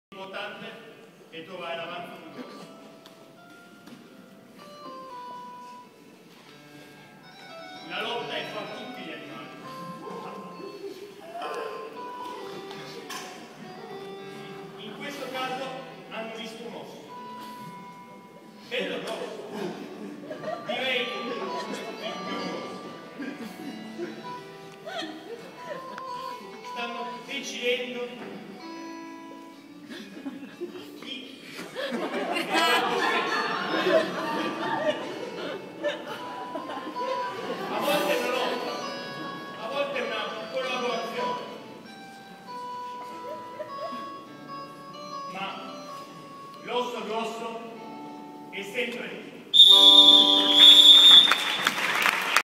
IMPROVVISUMMER 2012 - Match primi anni 2
Match serale dei primi anni in occasione degli stages di Improvvisummer 2012 presso Principina, Grosseto, Italia